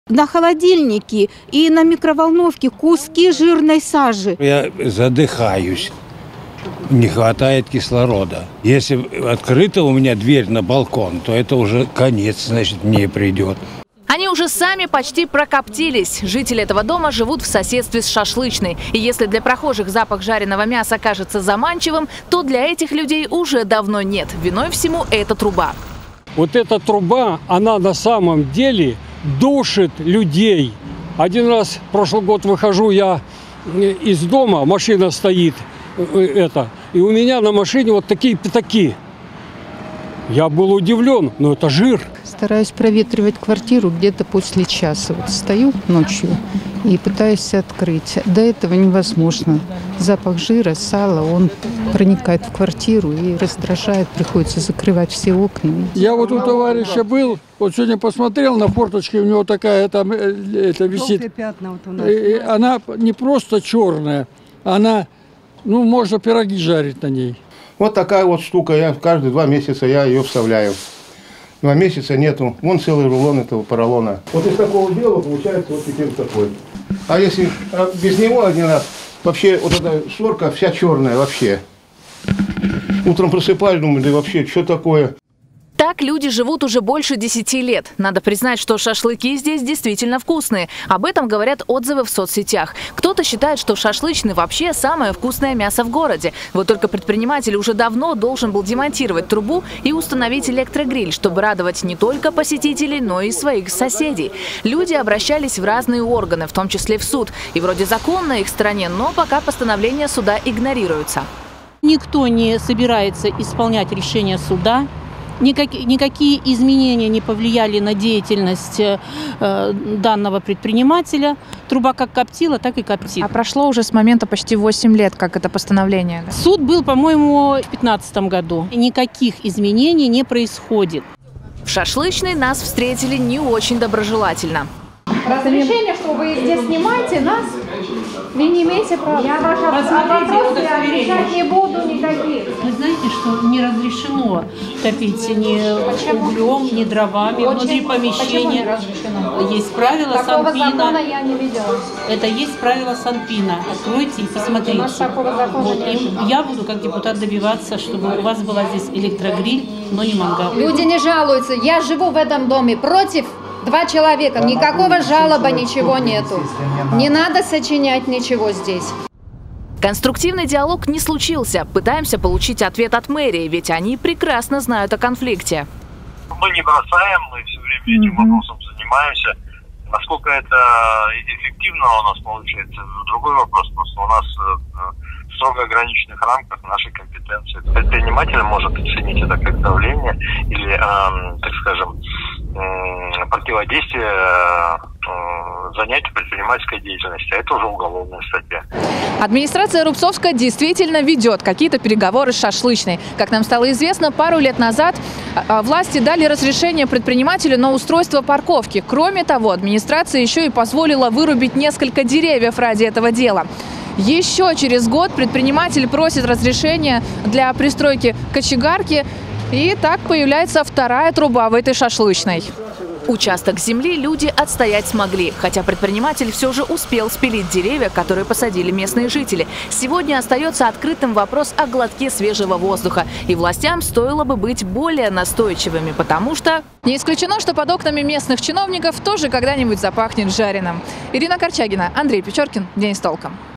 Шашлычная в Рубцовске "отравляет жизнь" жителям
Жители Рубцовска уже десять лет задыхаются из-за шашлыка. Люди живут по соседству с шашлычной, которая, как они говорят, отравляет им жизнь. Всё дело в трубе. Дым из неё окутывает ближайшие дома. Жители порой даже не могут открыть окна из-за постоянного запаха жареного мяса и сала. При этом, в местной администрации не могут решить эту проблему в рамках своей компетенции, опасаясь излишнего давления на предпринимателя. Подробнее в нашем репортаже.